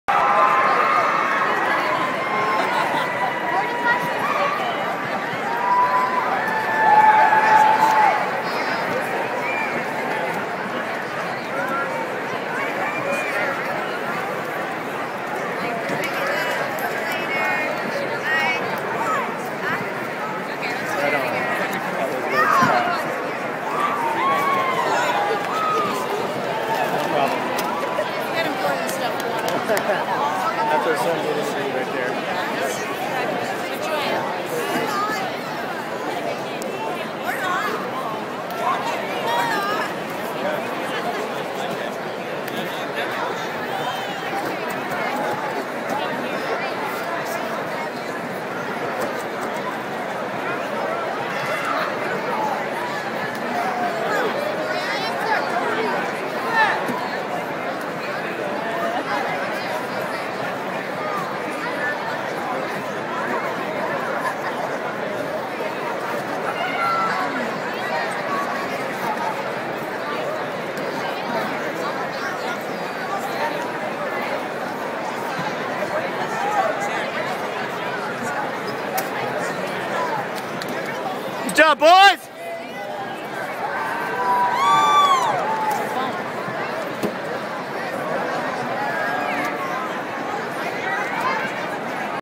Oliver Interview after Win
Durrell and Oliver interviewed after awesome Zillah win over Cashmere HS in semi finals for state!  Asked about what it's like to be part if such a great experience as a senior, etc.